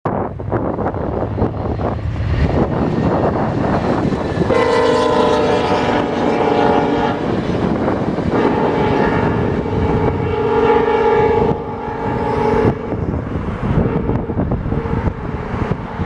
VIA No 64 Eng 907 at Kingston, ON on April 23 2016
VIA No 64, the afternoon 'baggage car train' whistles through Kingston's west end near Mi 179 Kingston Sub on April 23. Three ex-CP Canadian cars are in the consist: the baggage and two coaches. Minor wind noise.